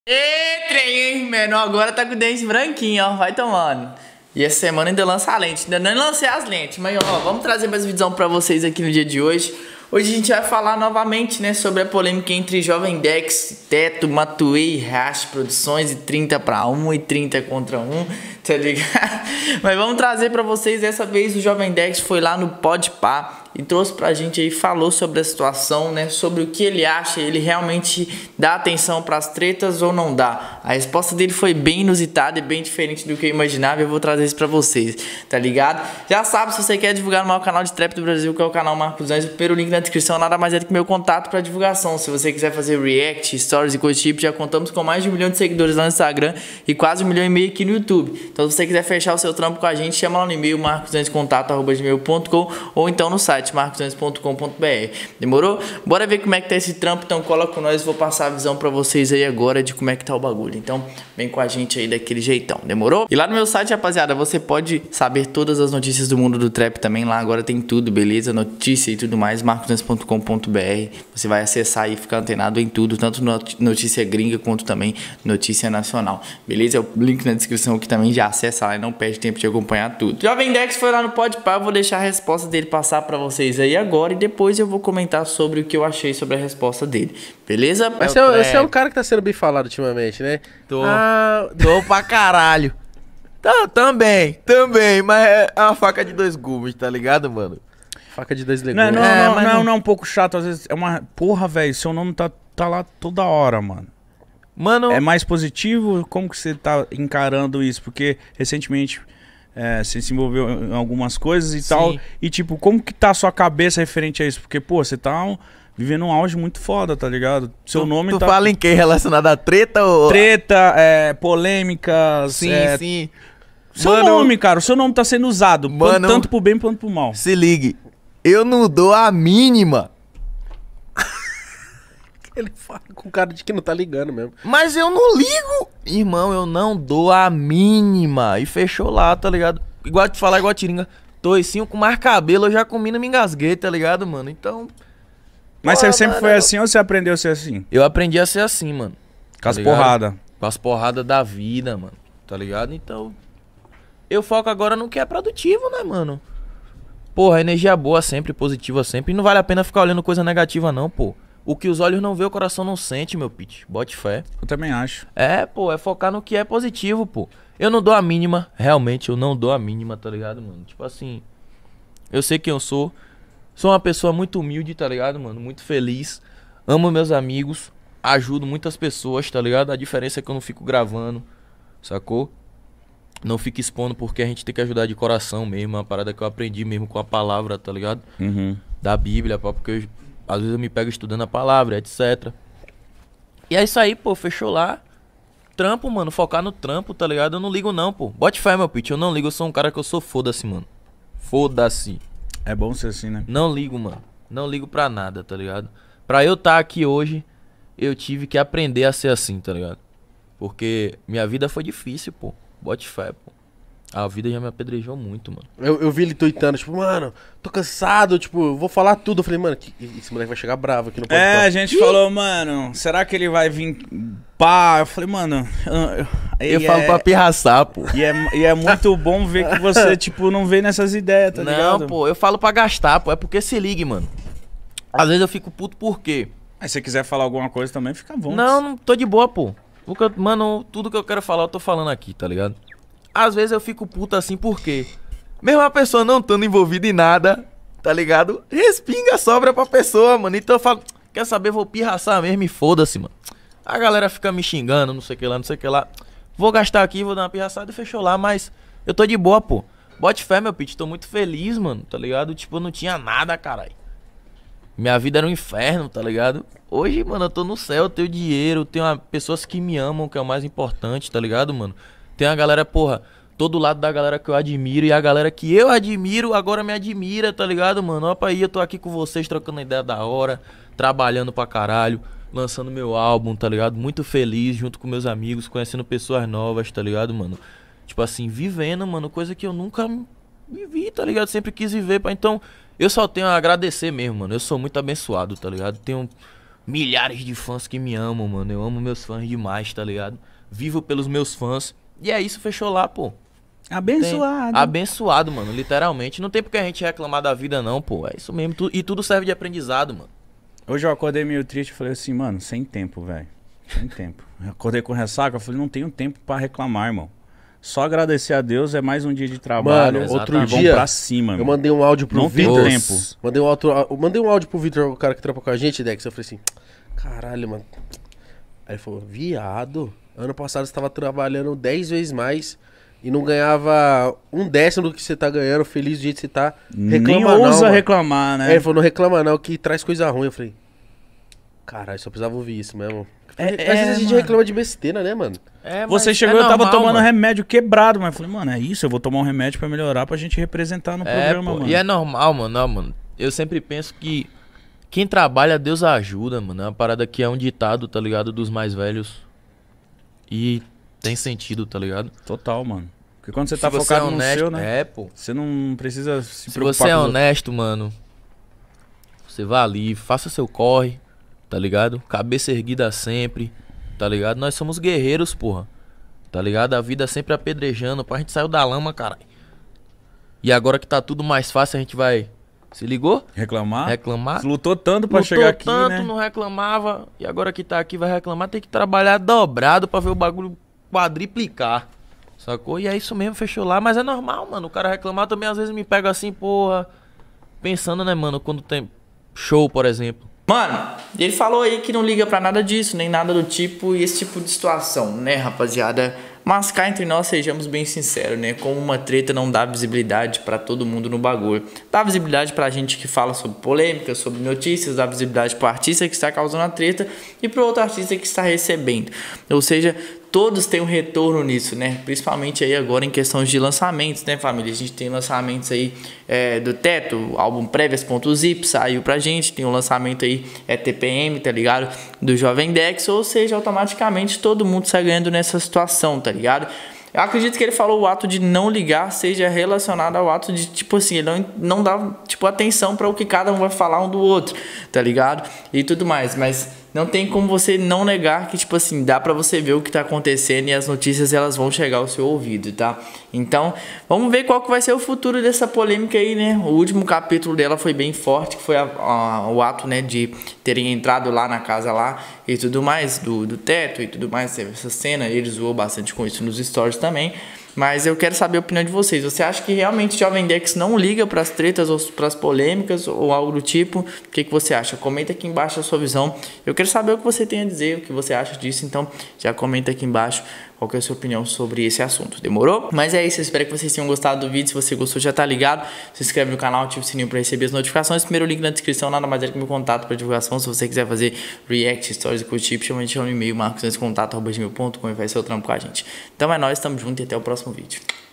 JOVEM DEX MANDA RECADO PRO TETO *não ligo pra ele*
Canal de RAP/TRAP e falo: Meno Tody, Matuê, Teto, NGC Daddy, Borges, Raffa Moreira, BC Raff.